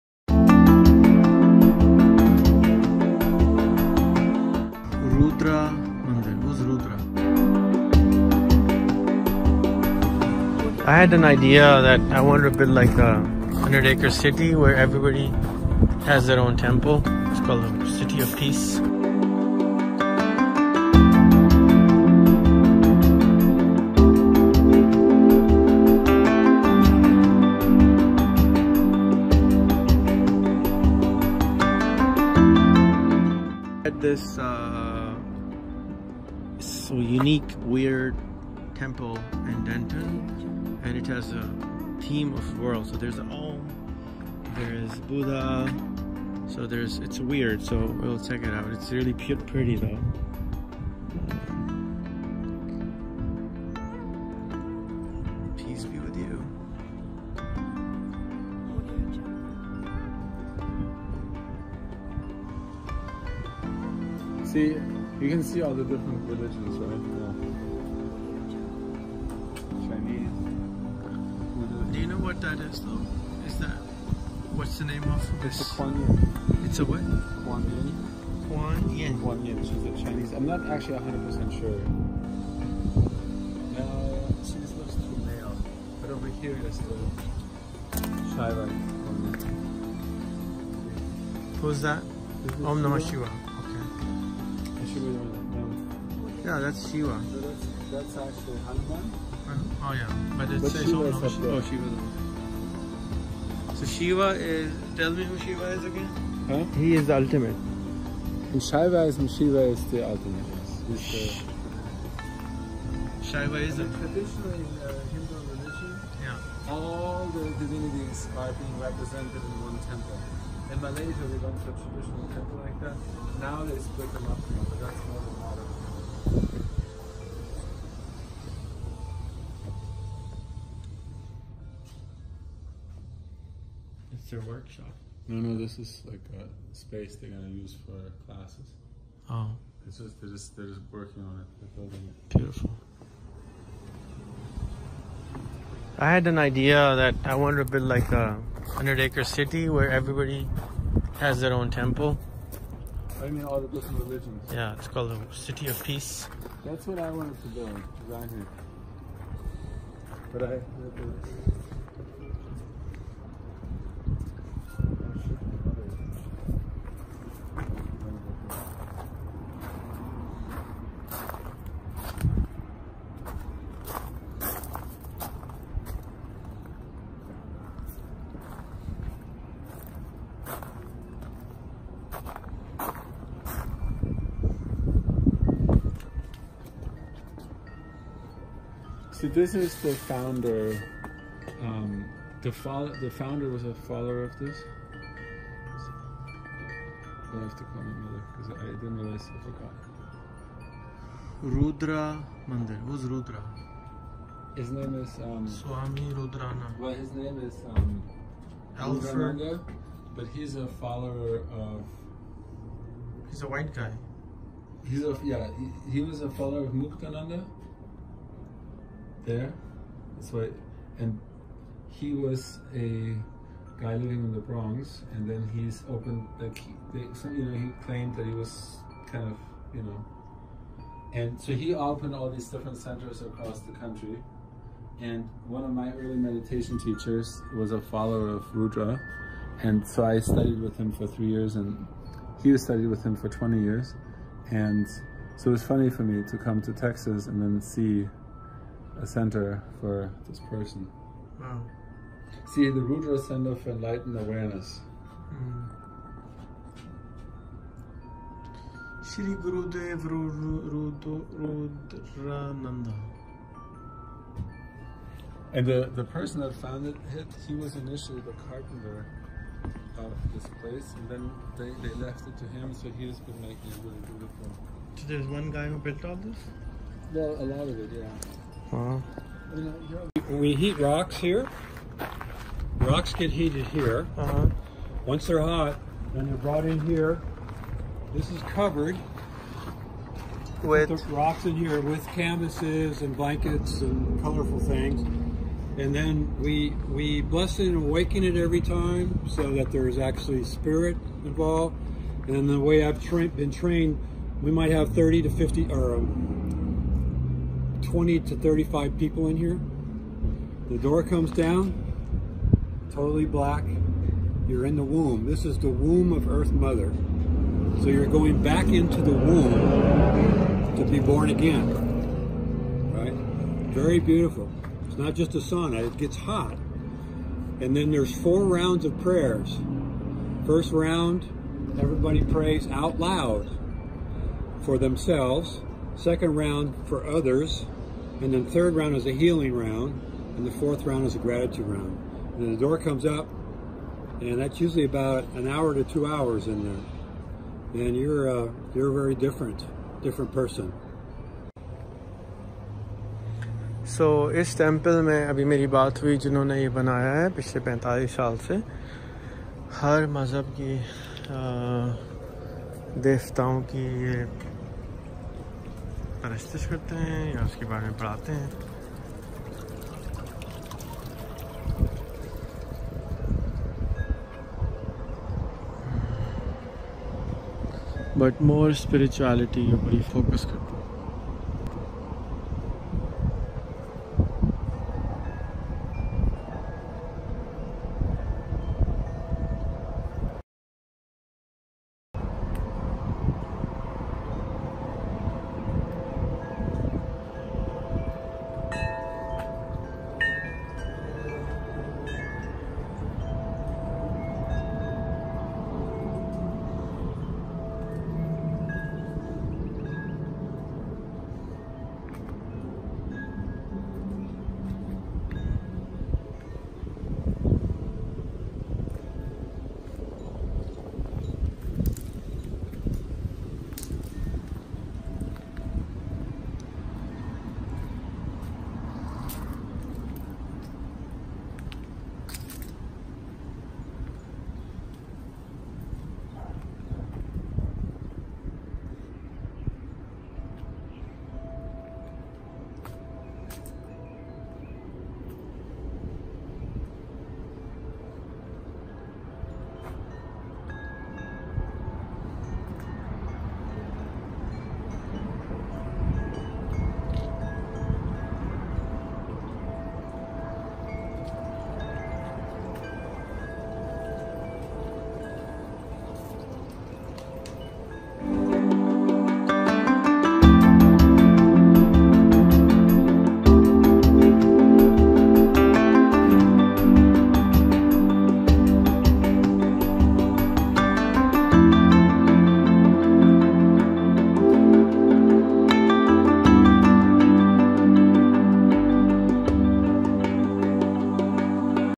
Rehan Visits RUDRA CENTER FOR ENLIGHTENED AWARENESS | Rehan Allahwala
A Place Where Spirit Takes Up Residence And Transforms All Pain And Suffering.
The Rudra Center for Enlightened Awareness is an assemblage that provides impactful Spiritual experiences for individuals seeking to find a higher meaning and purpose to their lives. It provides a nourishing environment for the journey towards profound physical and spiritual well-being. Its vision is to serve the community by providing innovative tools of Self-knowledge for effective and dynamic living expression. Offering, under one roof, a variety of spiritually focused programs to aid in the exploration of each individual’s full potential.

From ancient times the cultures of the world have turned to the transformational and healing effects of art, purification, self-knowledge and spiritual practices in the search for health, hope, happiness, and harmony.

Upholding that tradition, Rudra Center is pleased to stimulate more choices as to how one experiences themselves as a Vibrational Being and to make contact with Spirit through various programs that work – Rudra Meditation, Tai Chi & Qi Gong, NLP Training, Contemporary Shamanism, Self-Knowledge Classes, Spiritual Psychology, Shadow Mountain Yoga, Spiritual Clearance/ Purification, and The Way of the Disentangled Adventurer.

The experience of celebration and expansion at the Rudra Center is enhanced through the adornment of the space with sacred art in the form of sculpture, visionary painting, and architectural elements, evoking the magnificence of Spirit.